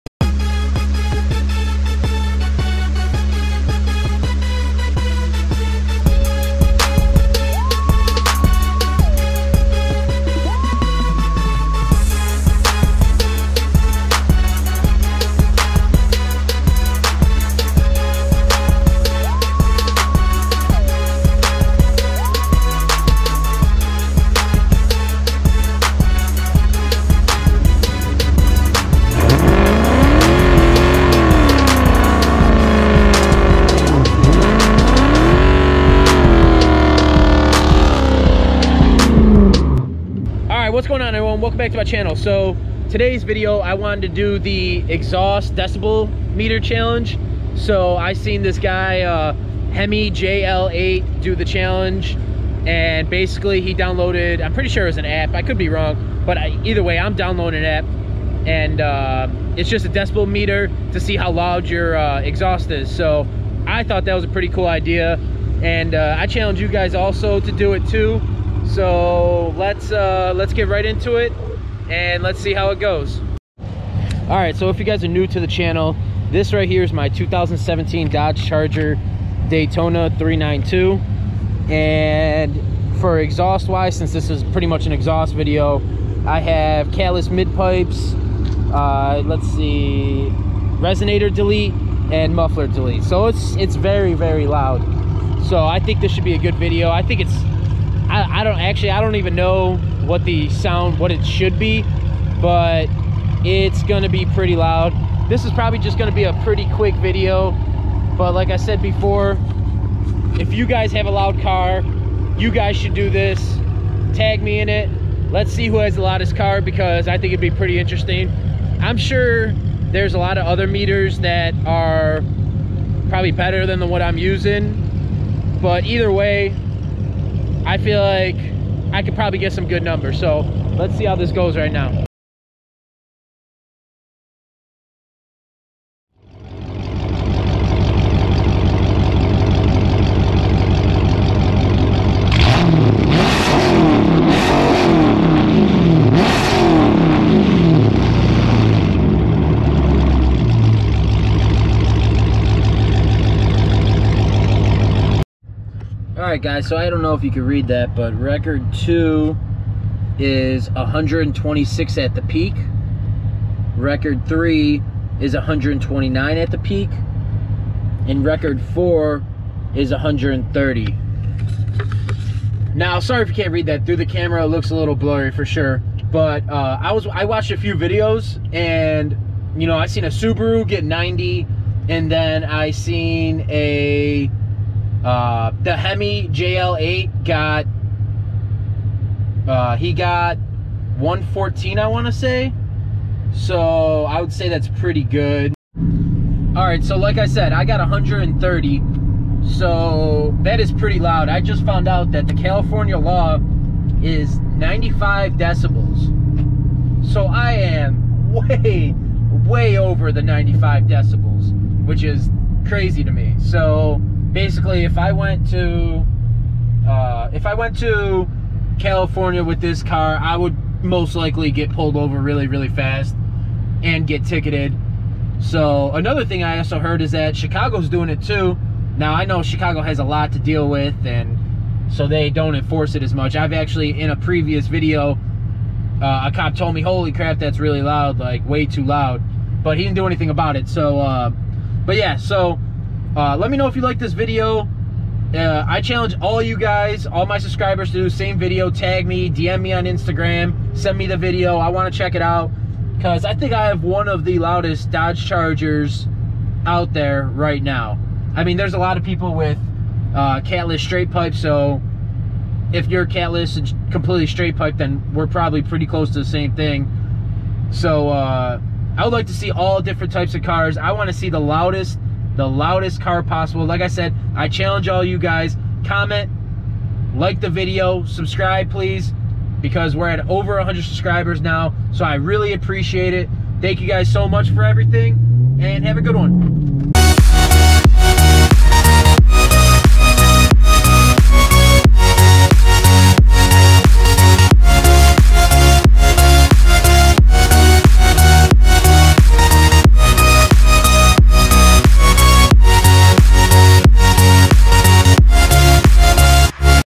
HOW MANY DECIBELS IS A CATLESS STRAIGHT PIPE SCAT PACK / 392!! DECIBEL CHALLENGE!
This was a fun video i wanted to make to see if my car is actually loud or not. this is a 2017 dodge charger 392 straight piped and catless! show some love on the video. I challenge everyone to make there own DB exhaust video and link me it!